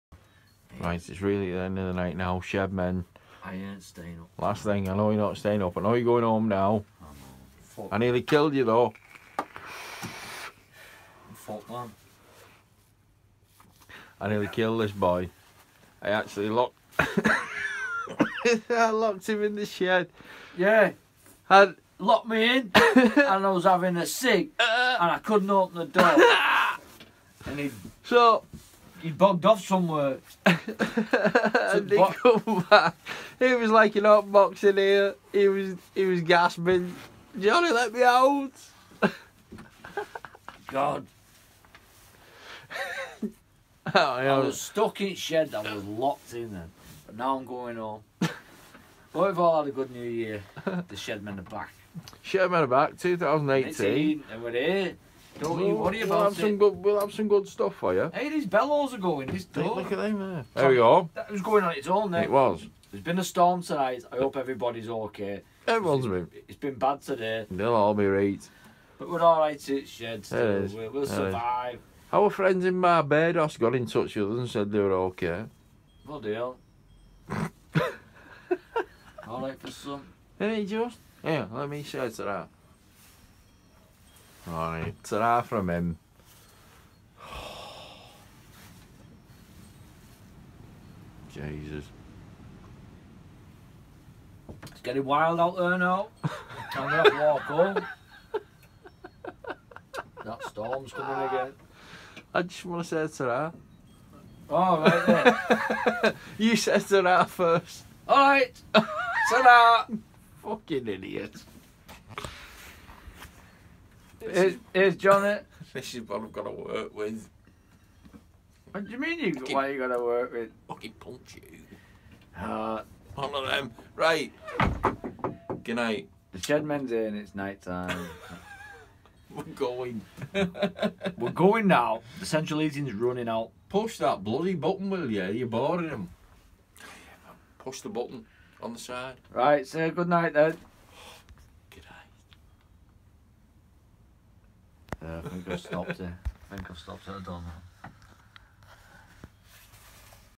The Shed Men...I didn't mean to nearly kill J!!
Locked J in the shed by accident, he decided to have a Rollie!!! Ha ha ! We also discover that Jay can't stop a video on an IPhone 😂😂✌️